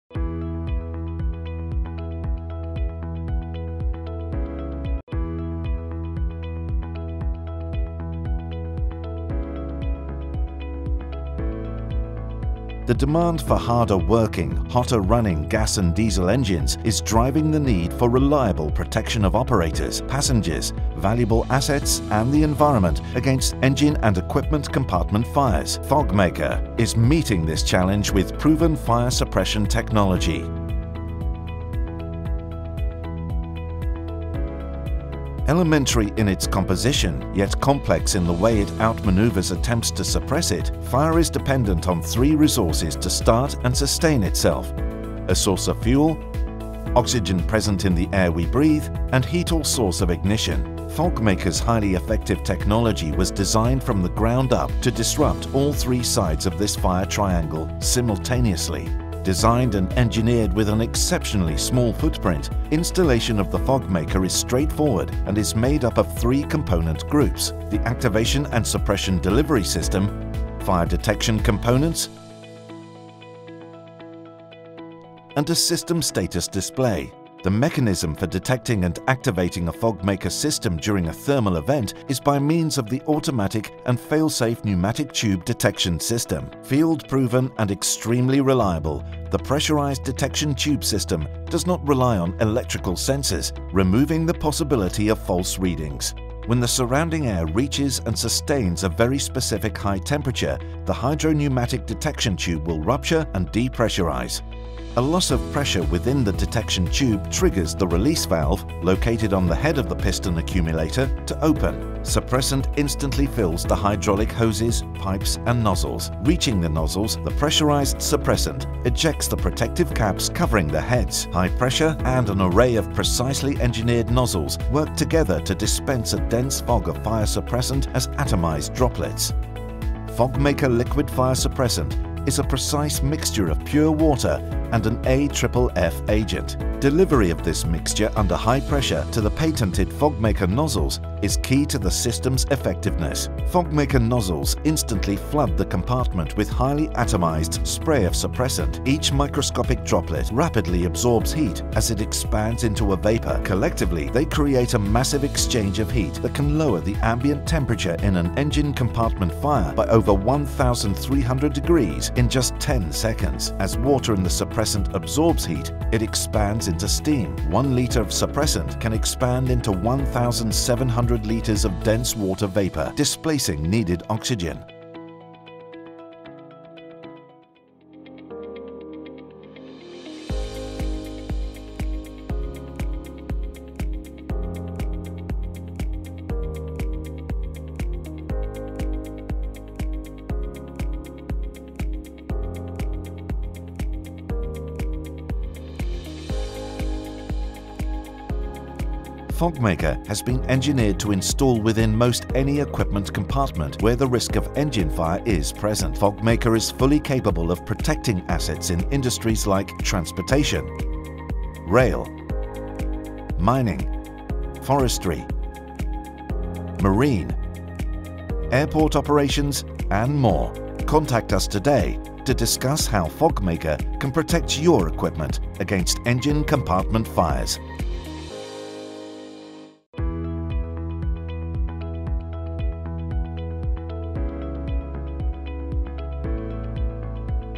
【製品紹介】FOGMAKER～ウォーターミスト消火システム～
エンジンルーム内の火災をウォーターミストで急速消火。
消火剤のベースは水ですので、人・製品・環境に無害。
建設重機（ホイールローダーや、油圧ショベル）・飛行機牽引車などでもご採用頂いております。